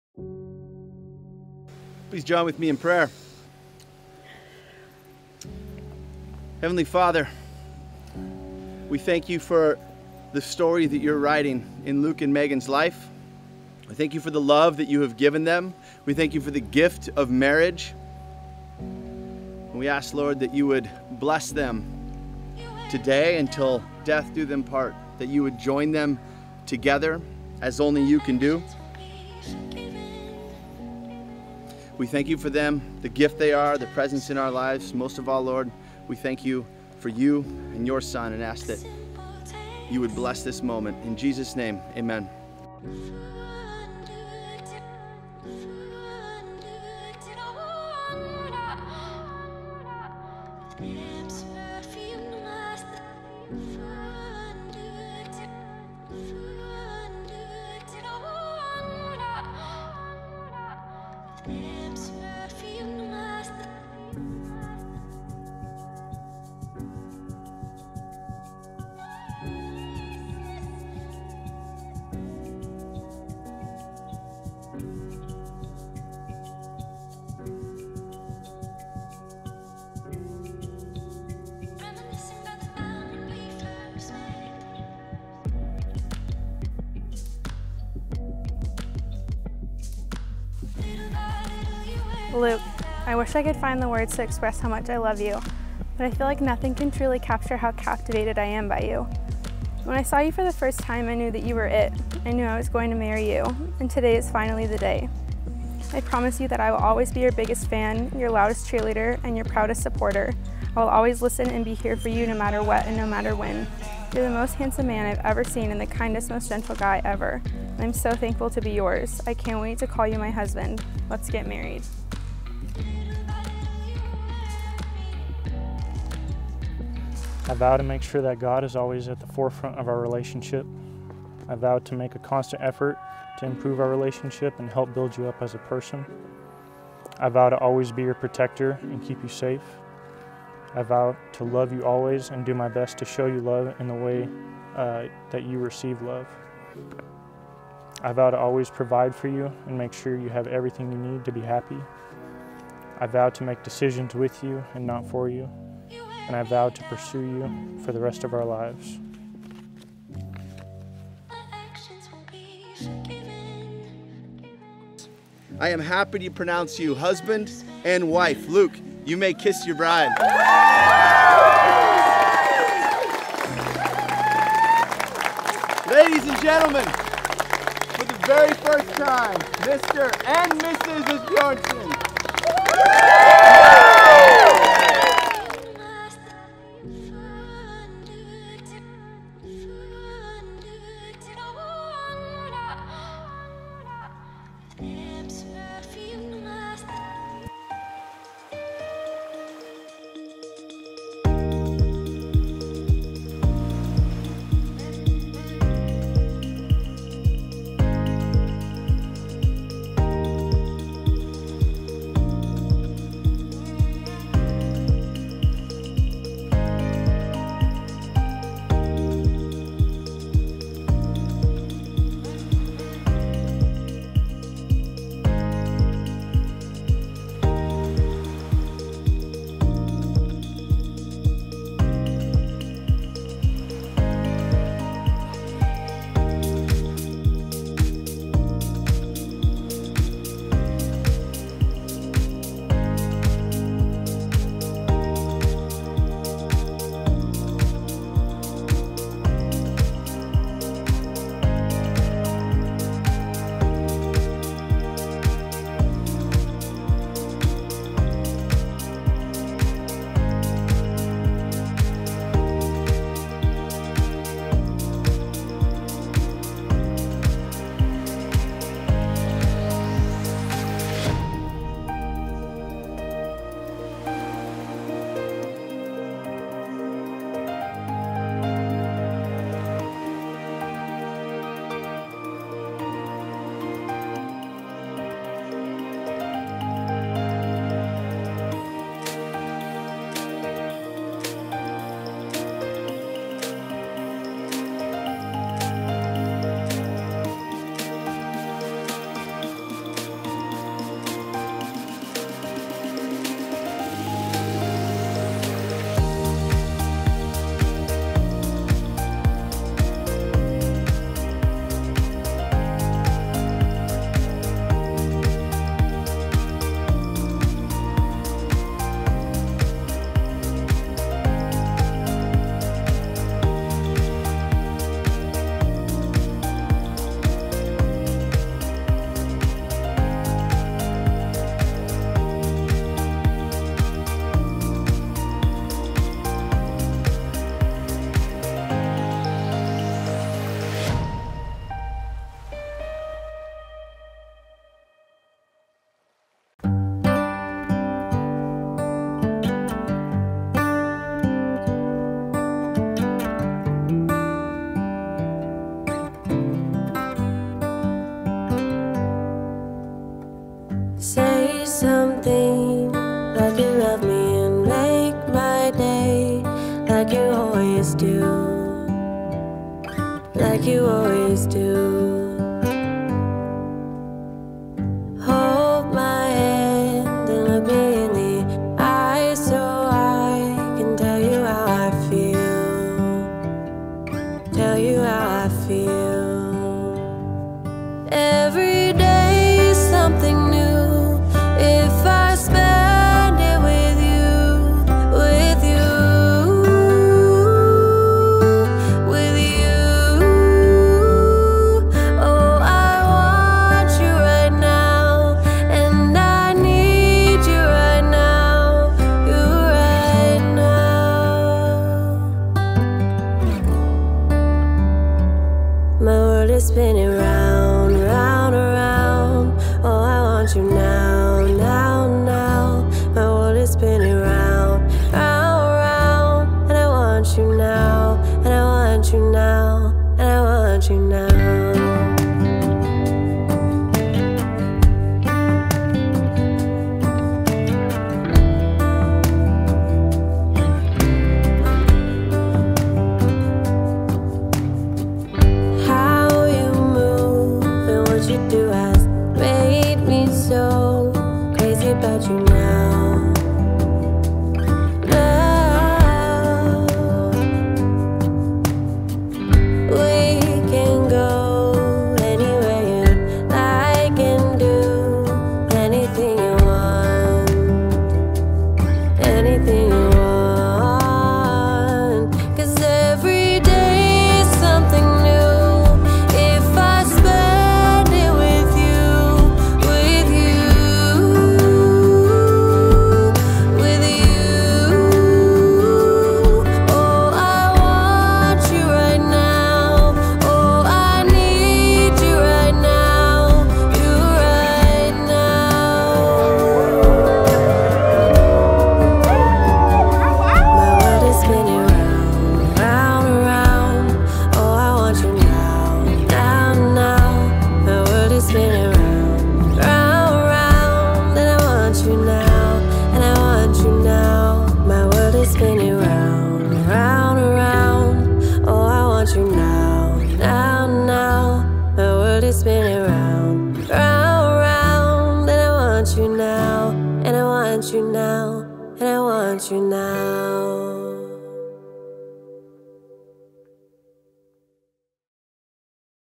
Asbjornsen Wedding | Chehalis, Washington
This cinematic feature documents Luke & Megan's riverside Wedding in the rural Chehalis, Washington.